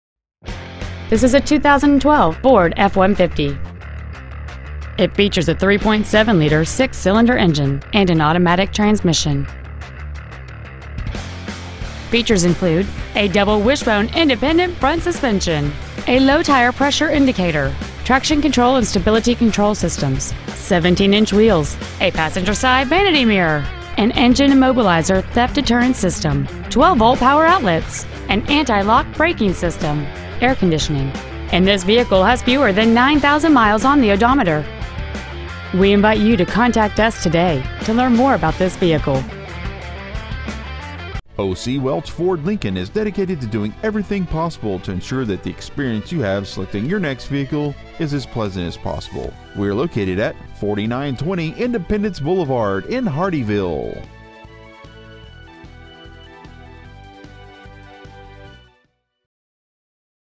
Pre-Owned 2012 FORD F-150 Hardeeville SC
Year : 2012
 Make : FORD
 Model : F-150
 Engine : 3.7L V6 24V MPFI DOHC FLEXIBLE FUEL
 Trans . : 6-SPEED AUTOMATIC
 Exterior : OXFORD WHITE
 Miles : 8,896

 Stock : 00P71464